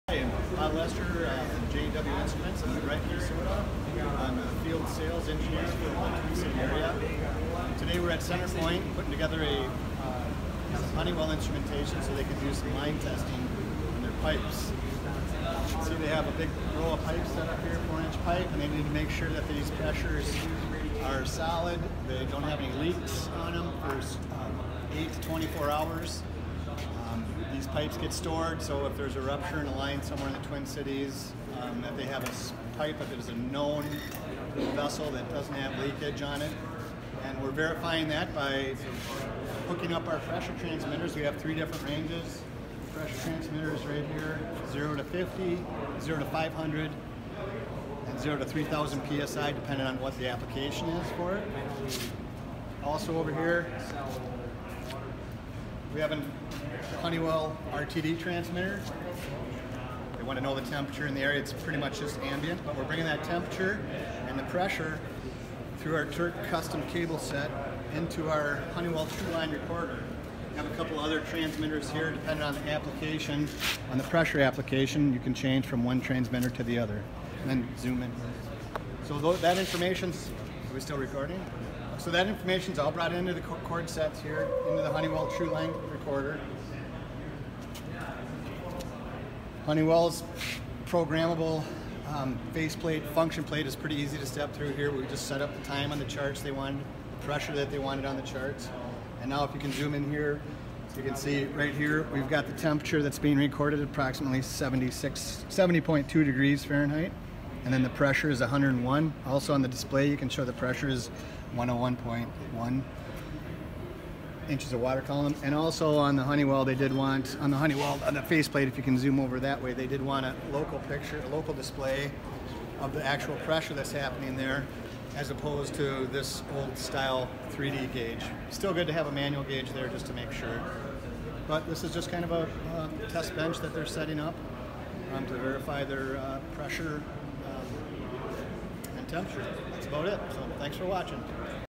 Pipeline test with Honeywell DR45 Recorder/Smartline Pressure Transmitter
J&W's Field Sales Engineer- Bob Lester at CenterPoint Energy performing a pipeline test using Honeywell DR45 Truline Recorder and Honeywell Smartline Pressure Transmitter.